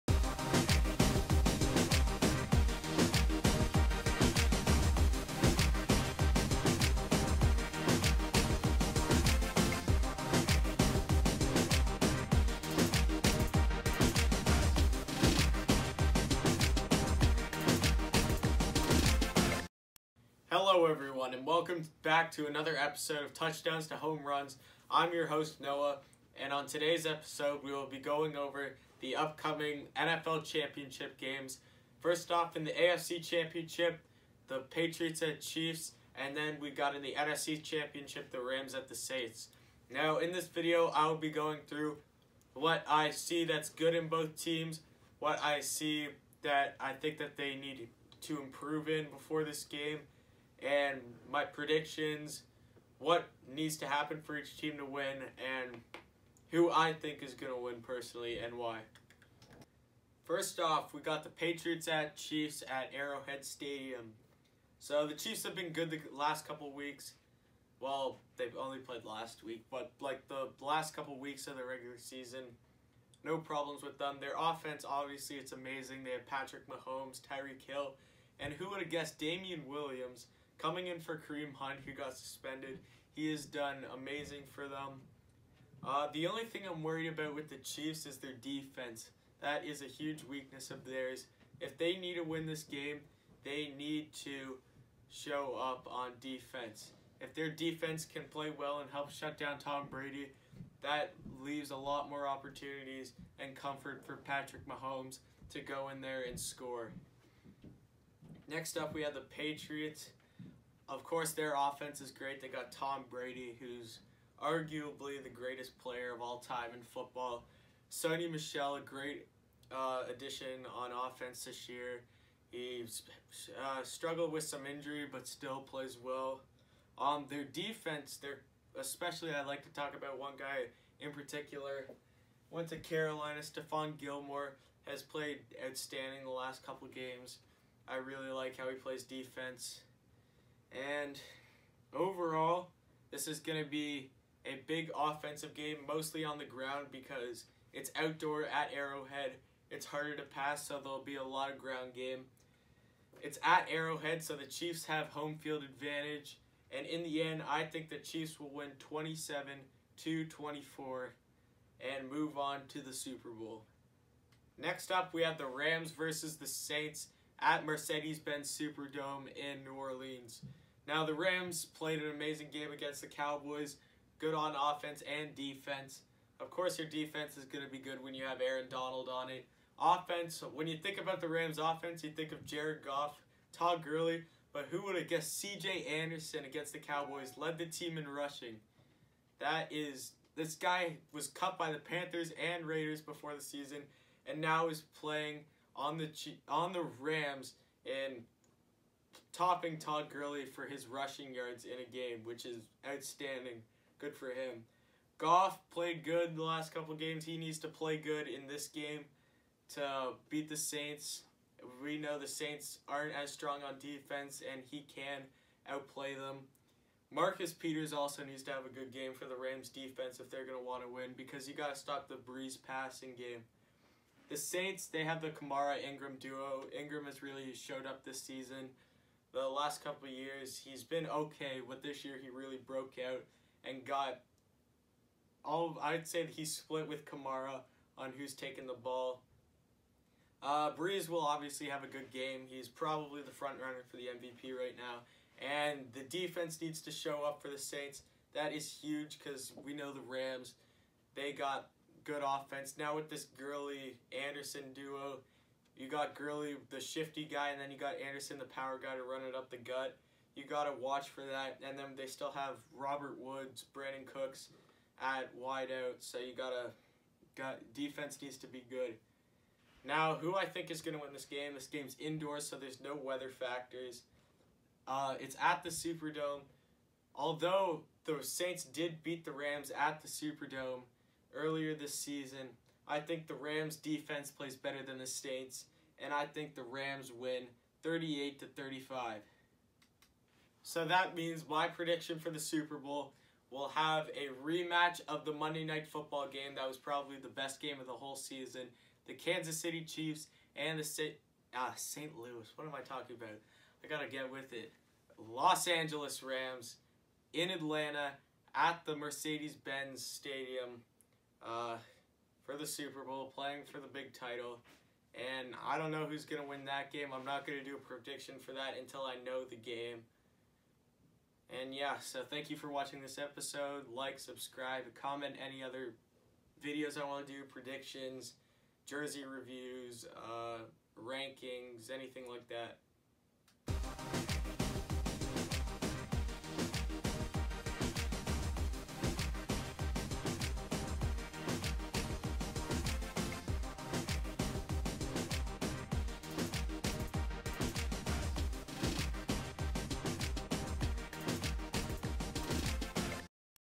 NFL Championship Games Predictions
On this episode I predicted the outcomes of each of the NFL championship games happening this Sunday January 20th 2019. This is just my opinion on what will happen.